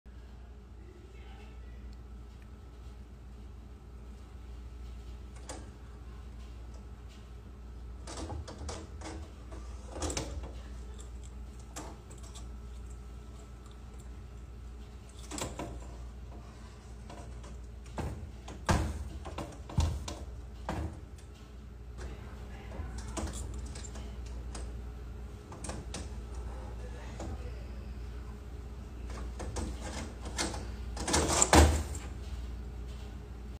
Monitor lizard attempts to break into house | USA TODAY
A Florida homeowner spotted a huge monitor lizard trying to get into his house.